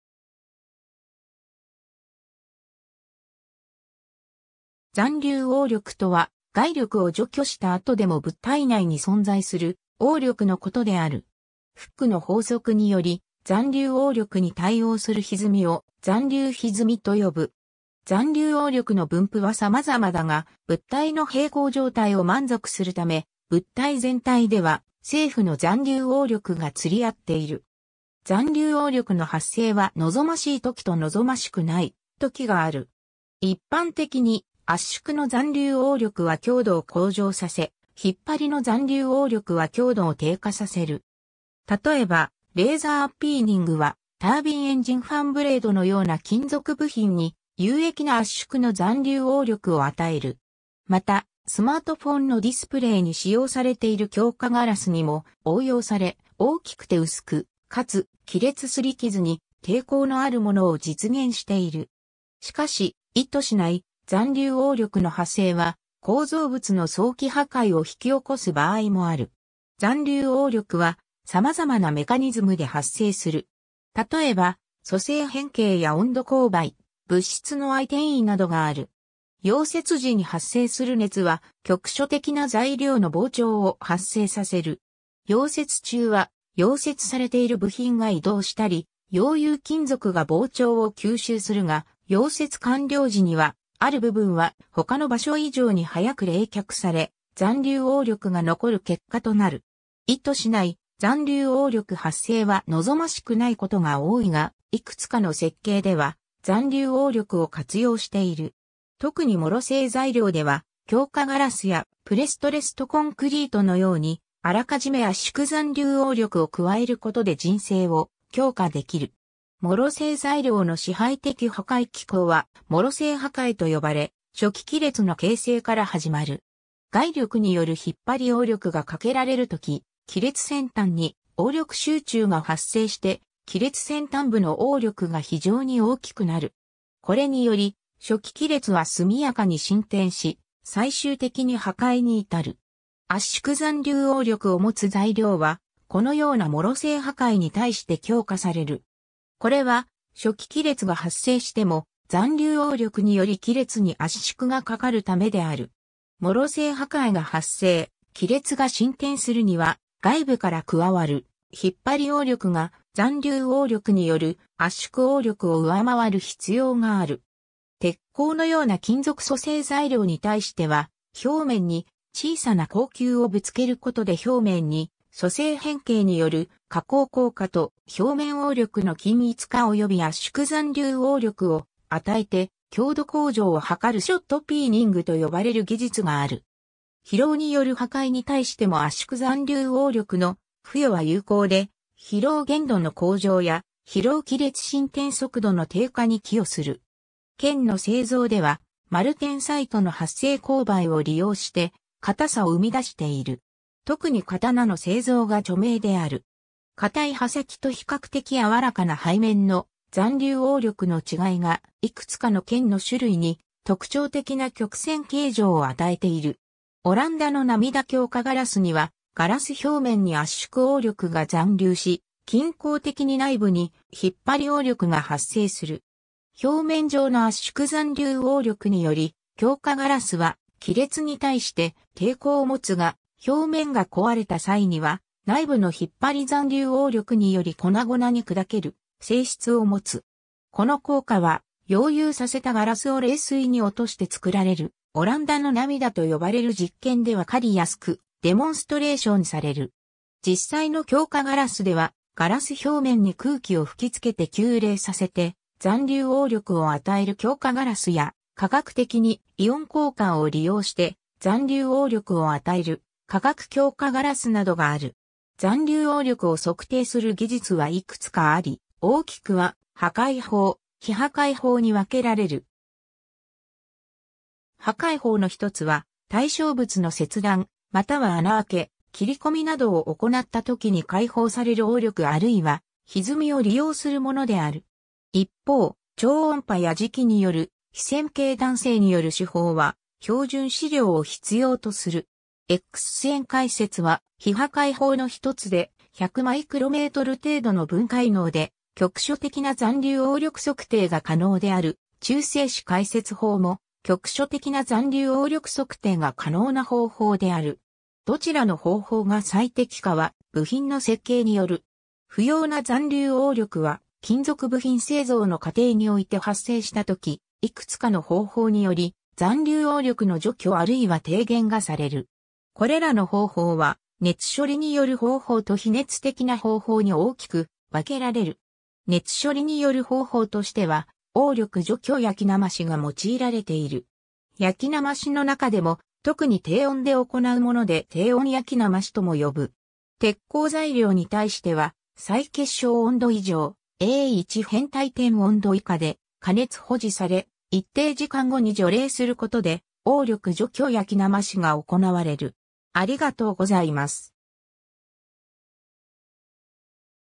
残留応力
残留応力 （ざんりゅうおうりょく、residual stress）とは、 外力を除去した後でも物体内に存在する応力のことである。
フックの法則により残留応力に対応するひずみを、残留ひずみ（ざんりゅうひずみ、residual strain）と呼ぶ。
残留応力の分布は様々だが、物体の平衡状態を満足するため、物体全体では正負の残留応力が釣り合っている。
 残留応力の発生は望ましいときと望ましくないときがある。
一般的に、圧縮の残留応力は強度を向上させ、引張の残留応力は強度を低下させる。
例えば、レーザーピーニングはタービンエンジンファンブレードのような金属部品に有益な圧縮の残留応力を与える。
また、スマートフォンのディスプレイに使用されている強化ガラスにも応用され、大きくて薄く、かつ、き裂・擦り傷に抵抗のあるものを実現している。
しかし、意図しない残留応力の発生は構造物の早期破壊を引き起こす場合もある。
 残留応力は様々なメカニズムで発生する。
例えば、塑性変形や温度勾配、物質の相転移などがある。
溶接時に発生する熱は局所的な材料の膨張を発生させる。
溶接中は、溶接されている部品が移動したり、溶融金属が膨張を吸収するが、溶接完了時には、ある部分は他の場所以上に早く冷却され、残留応力が残る結果となる。
 意図しない残留応力発生は望ましくないことが多いが、いくつかの設計では残留応力を活用している。
特に脆性材料では、強化ガラスやプレストレスト・コンクリートのように、予め圧縮残留応力を加えることで靱性を強化できる。
脆性材料の支配的破壊機構は、脆性破壊と呼ばれ、初期き裂の形成から始まる。
外力による引張応力がかけられるとき、き裂先端に応力集中が発生してき裂先端部の応力が非常に大きくなる。
これにより、初期き裂は速やかに進展し、最終的に破壊に至る。
圧縮残留応力を持つ材料はこのような脆性破壊に対して強化される。
これは初期き裂が発生しても残留応力によりき裂に圧縮がかかるためである。
脆性破壊が発生、き裂が進展するには、外部から加わる引張応力が残留応力による圧縮応力を上回る必要がある。
 鉄鋼のような金属塑性材料に対しては、表面に小さな鋼球をぶつけることで表面に塑性変形による加工硬化と表面応力の均一化及び圧縮残留応力を与えて強度向上を図るショットピーニングと呼ばれる技術がある。
疲労による破壊に対しても圧縮残留応力の付与は有効で、疲労限度の向上や疲労き裂進展速度の低下に寄与する。
 剣の製造では、マルテンサイトの発生勾配を利用して硬さを生み出している。
特に刀の製造が著名である。
硬い刃先と比較的柔らかな背面の残留応力の違いが、いくつかの剣の種類に特徴的な曲線形状を与えている。
 オランダの涙 強化ガラスには、ガラス表面に圧縮応力が残留し、均衡的に内部に引張応力が発生する。
表面上の圧縮残留応力により強化ガラスはき裂に対して抵抗を持つが、表面が壊れた際には内部の引張残留応力により粉々に砕ける性質を持つ。
この効果は、溶融させたガラスを冷水に落として作られるオランダの涙と呼ばれる実験で分かり易くデモンストレーションされる。
 実際の強化ガラスでは、ガラス表面に空気を吹き付けて急冷させて残留応力を与える強化ガラスや、化学的にイオン交換を利用して残留応力を与える化学強化ガラスなどがある。
 残留応力を測定する技術はいくつかあり、大きくは破壊法、非破壊法に分けられる。
破壊法の1つは、対象物の切断または穴あけ、切り込みなどを行ったときに解放される応力あるいはひずみを利用するものである。
一方、超音波や磁気による非線形弾性による手法は標準試料を必要とする。
 X線回折 は非破壊法の1つで、100マイクロメートル程度の分解能で局所的な残留応力測定が可能である 中性子回折法 も局所的な残留応力測定が可能な方法である。
どちらの方法が最適かは部品の設計による。
 不要な残留応力は金属部品製造の過程において発生したとき、いくつかの方法により残留応力の除去あるいは低減がされる。
これらの方法は、熱処理による方法と非熱的な方法に大きく分けられる。
 熱処理による方法としては、応力除去焼きなまし(stress relief annealing)が用いられている。
焼きなましの中でも、特に低温で行うもので低温焼きなましとも呼ぶ。
鉄鋼材料に対しては再結晶温度以上、A1変態点温度以下で加熱保持され、一定時間後に徐冷することで応力除去焼きなましが行われる。